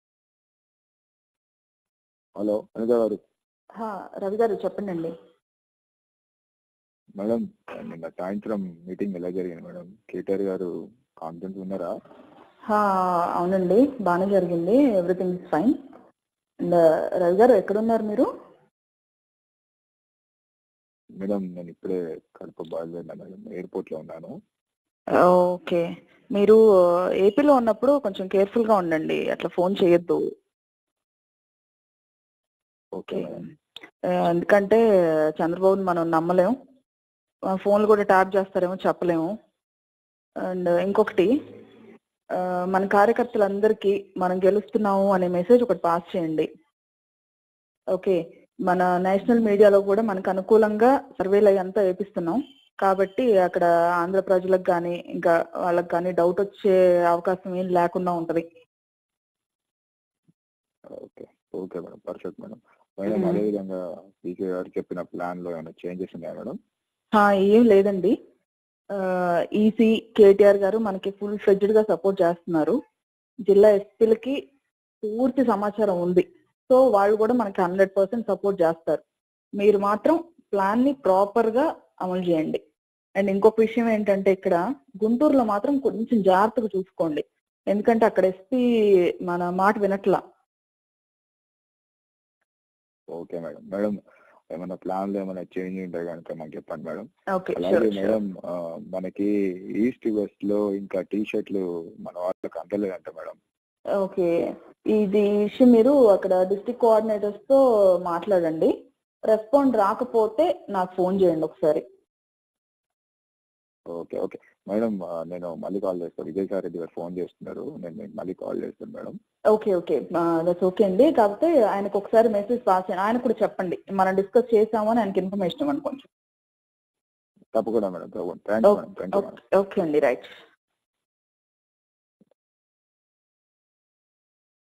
వైసిపి లీడర్ ప్రశాంత్ కిశోర్ ఆడియో లీక్ చంద్రబాబు పవన్ కల్యాణ్ కు ఏమి అనారో వినండి
Any questions please drop in comment section

This video only educational purposes